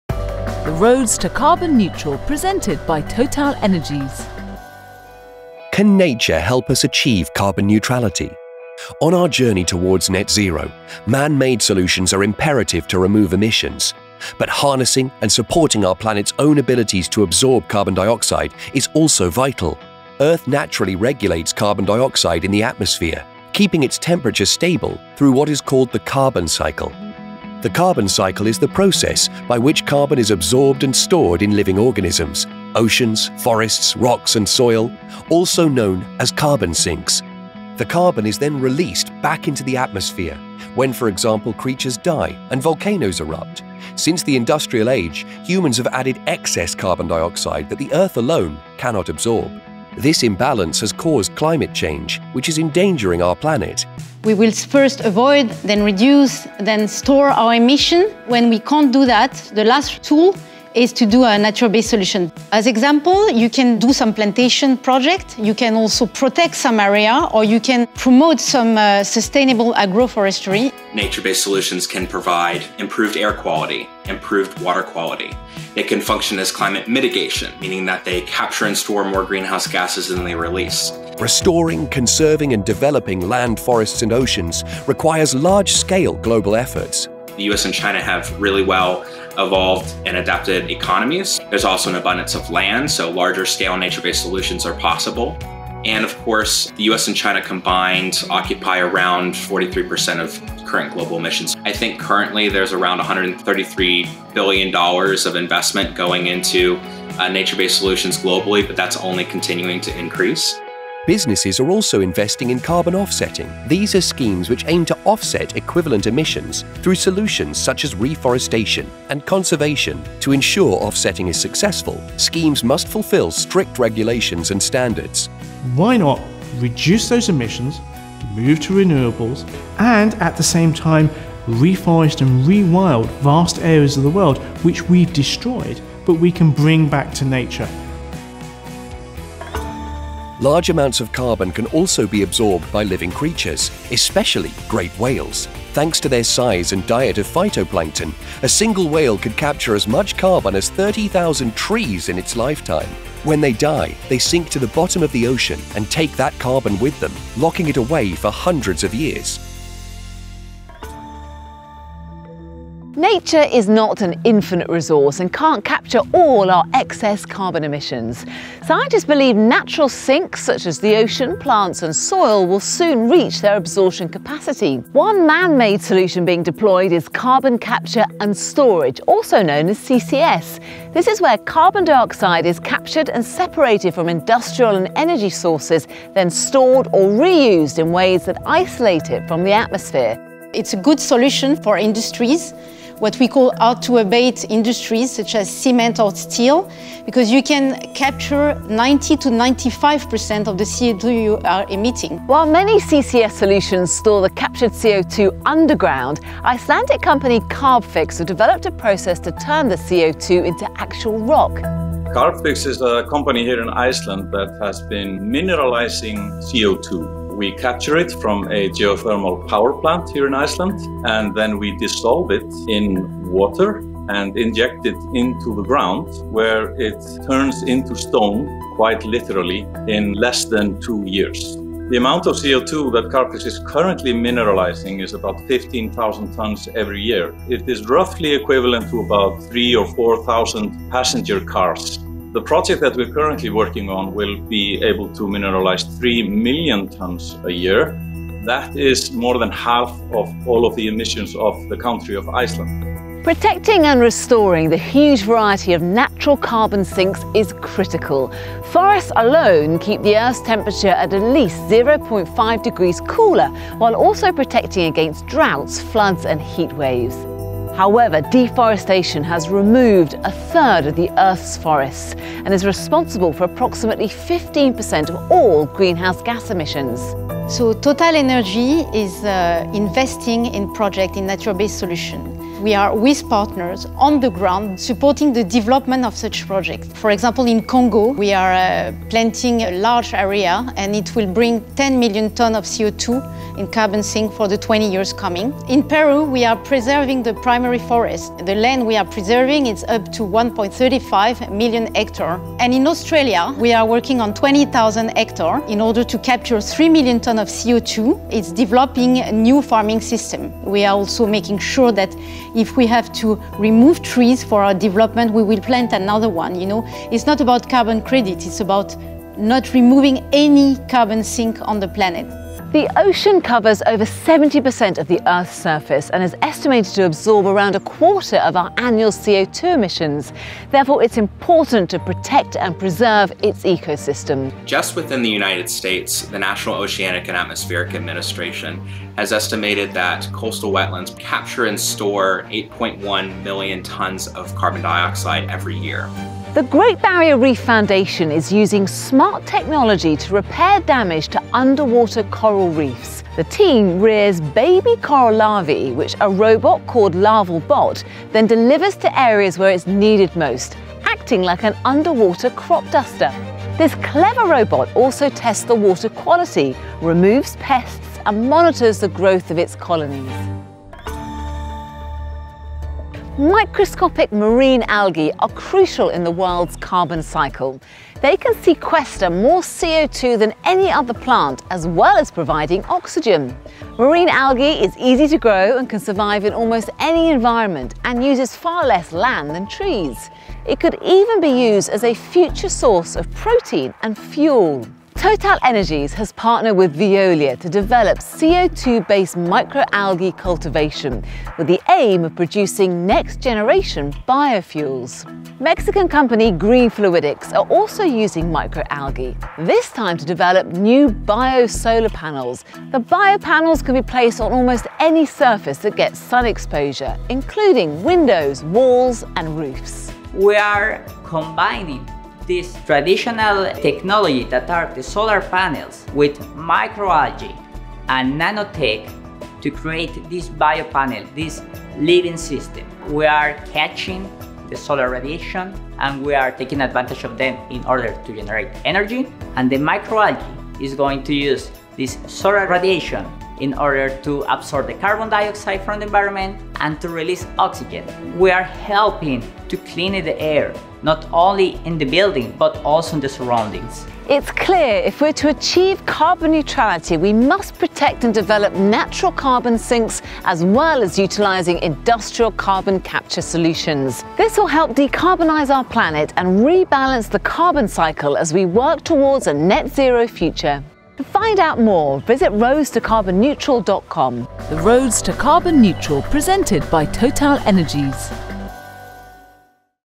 Can nature help us to achieve carbon neutrality?
Did you know that each great whale sequesters 33 tons of CO2 every year? Learn more about the role of nature in absorbing excess emissions.

Paid post by  @TotalEnergies